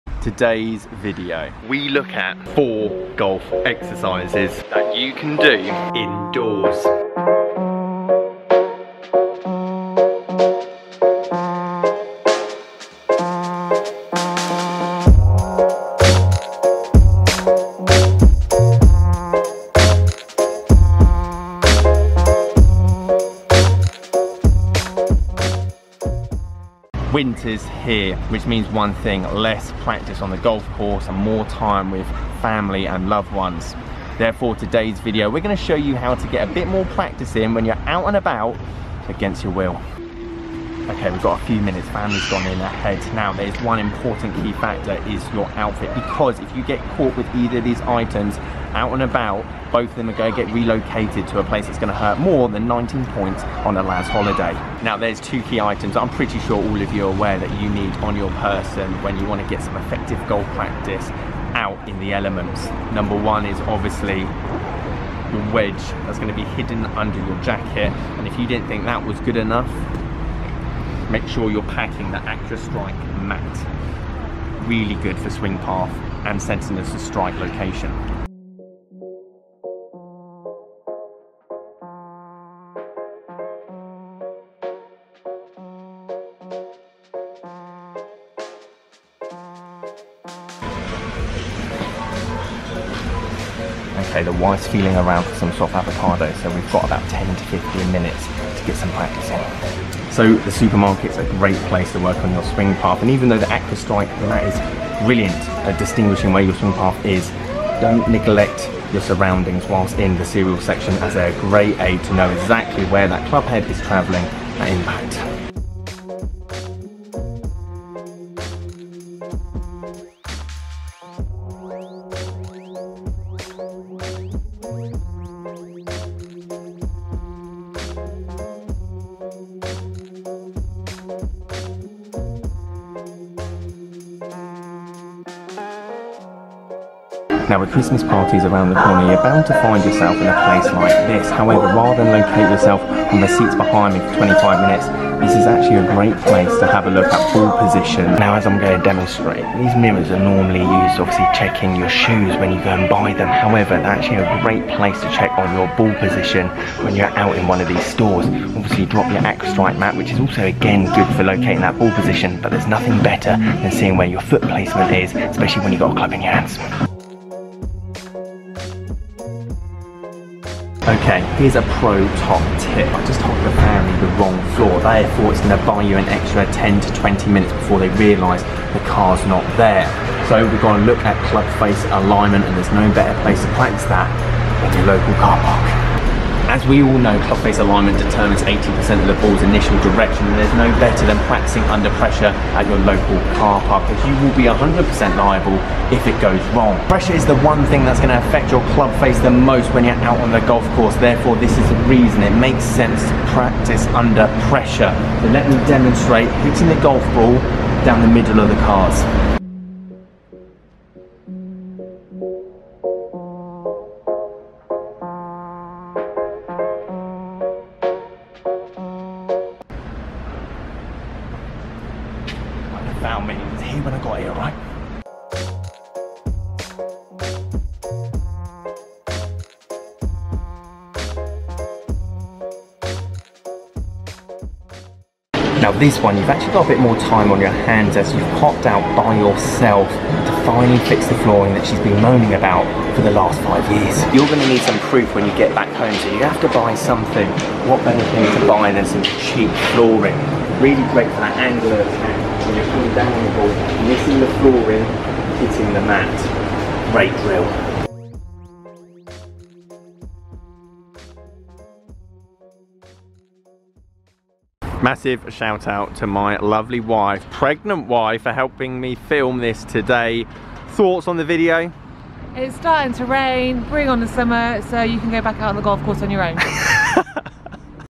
HOW TO PRACTICE YOUR GOLF SWING THROUGH WINTER
We take a look at 4 ways of practicing your golf swing throughout the winter period whilst out with the family and not getting caught.

Didn't take any commission again boys so if you fancy a mat link down below ;)

CODE: SAS GOLF for £10 off

Location:
Burford Golf Club, UK